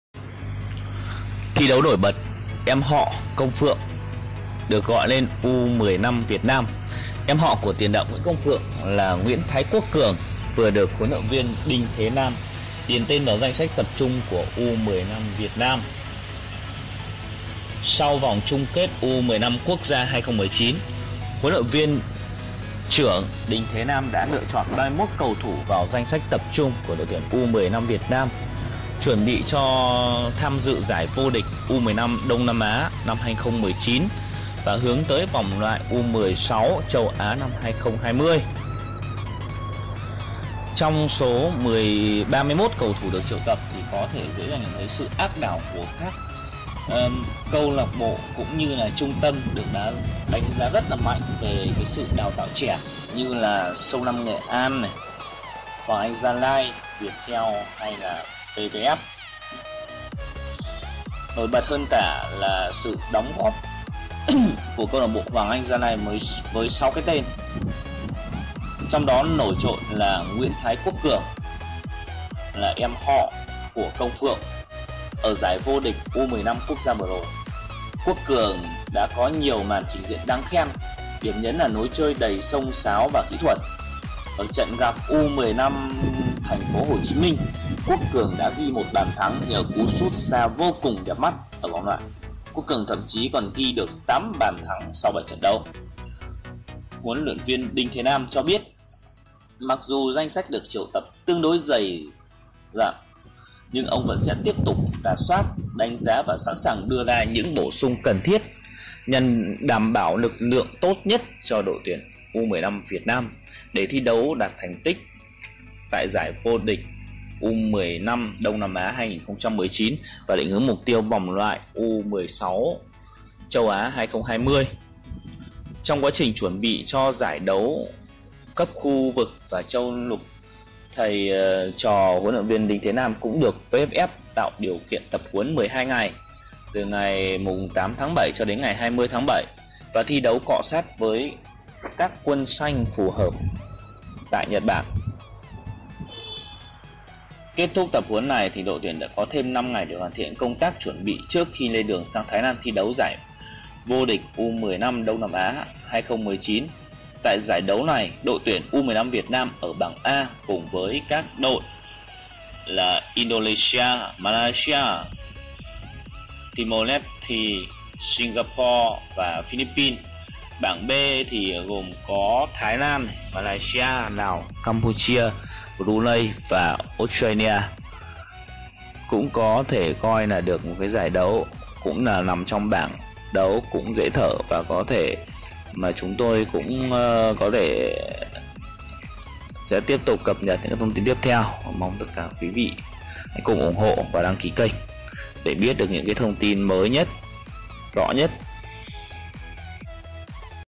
Em họ của Công Phượng - Nguyễn Thái Quốc Cường được gọi lên U15 Quốc Gia
Em họ của tiền đạo Nguyễn Công Phượng là Nguyễn Thái Quốc Cường vừa được HLV Đinh Thế Nam điền tên vào danh sách tập trung U15 Việt Nam.
Nguyễn Thái Quốc Cường. Ở giải vô địch U15 Quốc gia vừa rồi, Quốc Cường đã có nhiều màn trình diễn đáng khen, điểm nhấn là lối chơi đầy xông xáo và kỹ thuật.
Nguyễn Thái Quốc Cường
Công Phượng